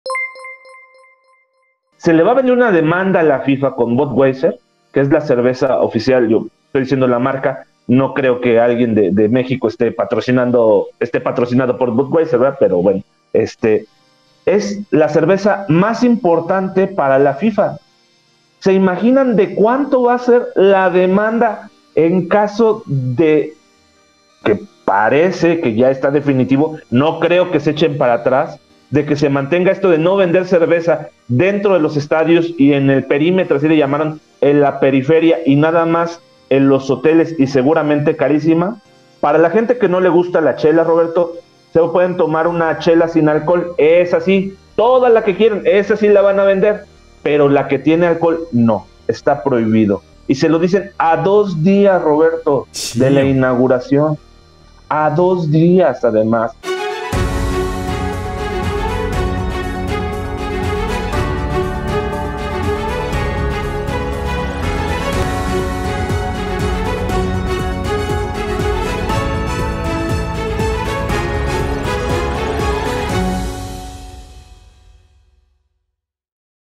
Qatar prohíbe la venta de cerveza en los estadios y sus alrededores durante el Mundial
Las autoridades de Qatar y de la FIFA anunciaron que no se venderá cerveza alrededor de los estadios durante el Mundial, en un comunicado publicado dos días antes del arranque de la competición.

【 Rᴇᴅᴇs Sᴏᴄɪᴀʟᴇs 】